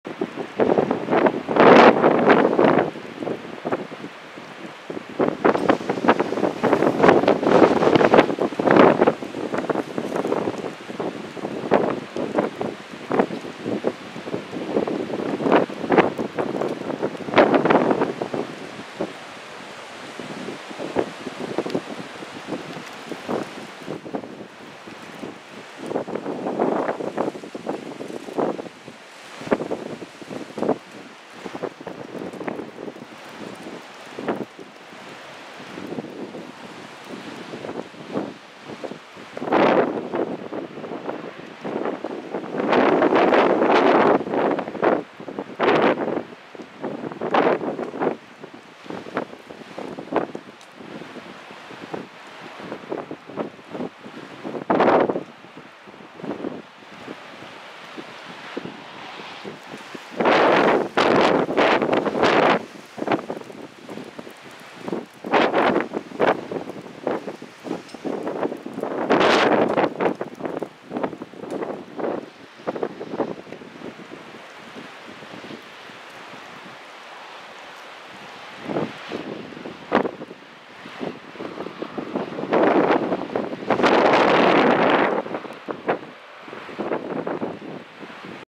Ekstremværet Gyda
Begynte i går å skal vare til i kveld eller tidlig fredags morgen. Dette er bare en smakebit av det vi får senere i kveld.